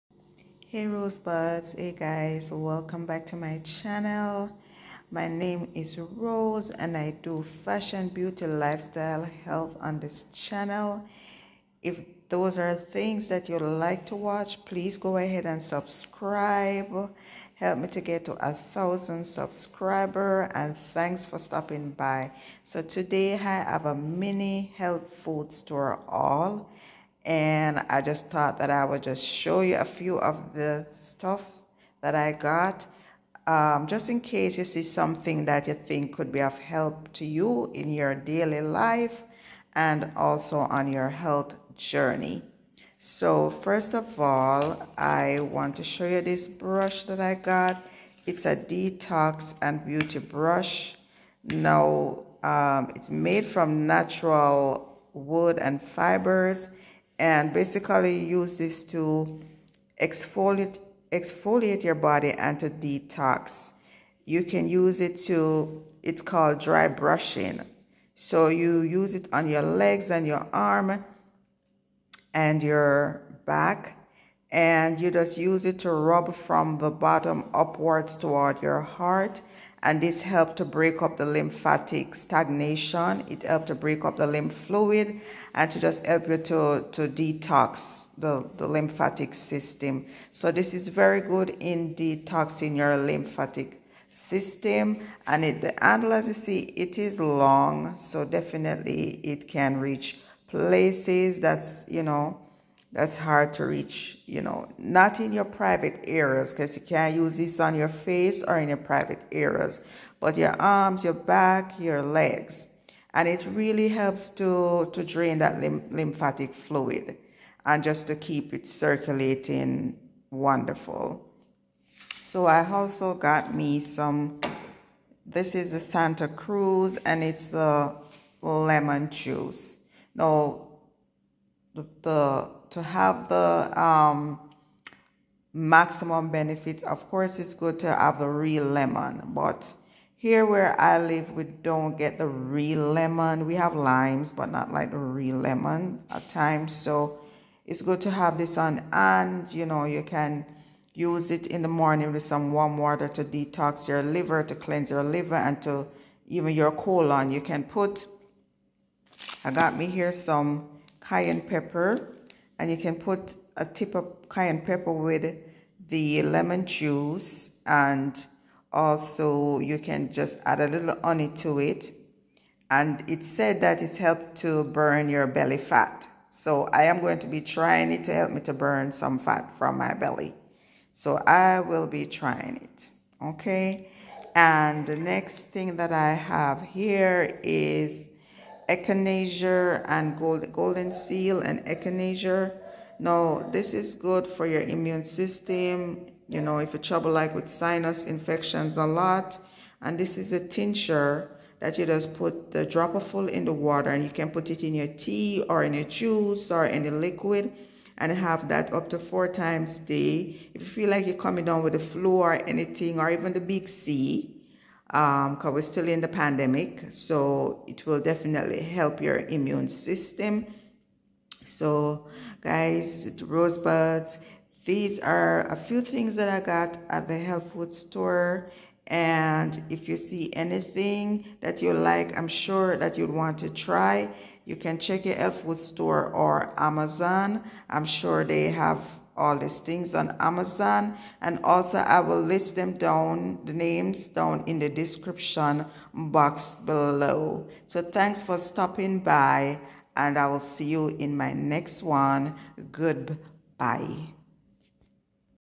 Health Food Store Haul
Products  thst helps to detoxify your body and boost your immune system.